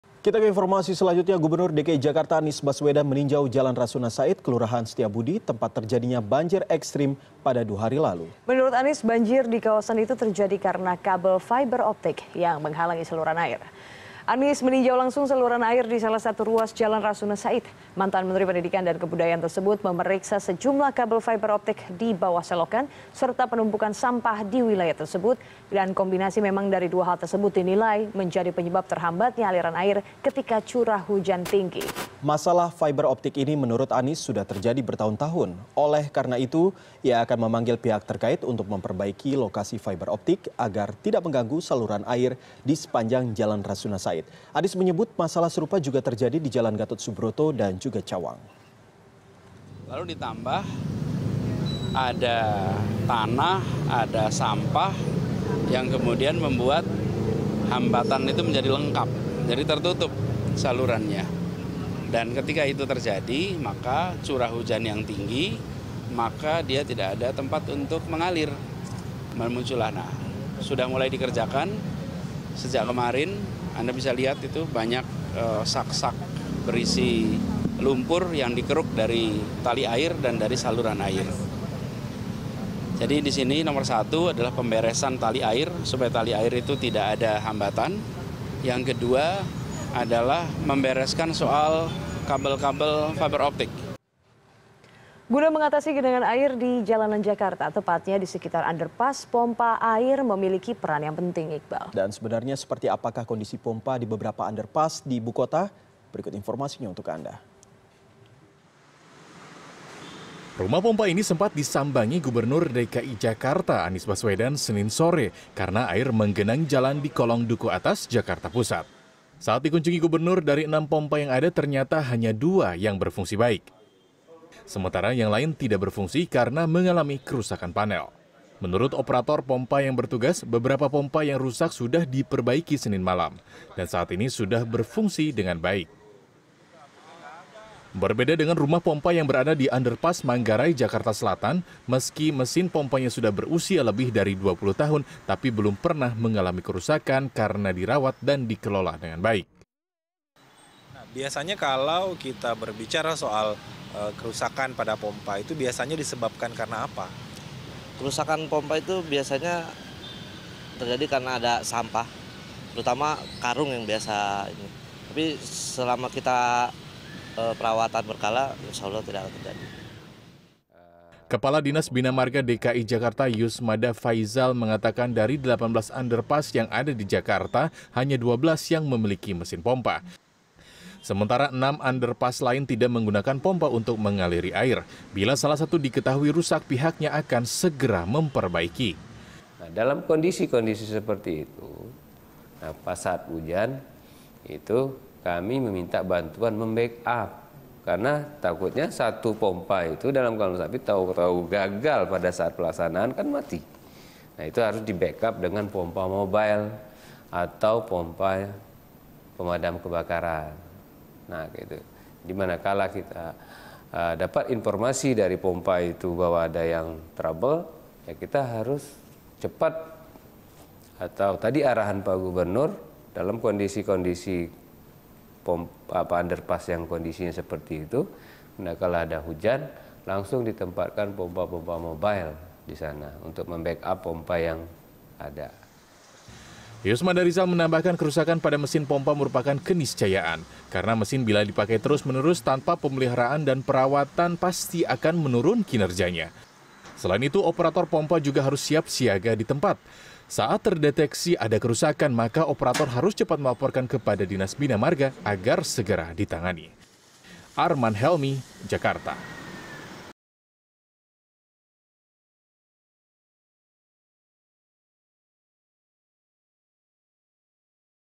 Anies Baswedan & Pemprov DKI Tinjau Penyebab Banjir Ibu Kota
Gubernur DKI Jakarta, Anies Baswedan meninjau jalan Rasuna Said, kelurahan Setiabudi, tempat terjadinya banjir ekstrem pada dua hari lalu. Menurut Anies Baswedan, banjir di kawasan itu terjadi karena kabel fiber optik yang menghalangi saluran air.

Like & Follow Facebook:
CNN Indonesia

cnnindonesia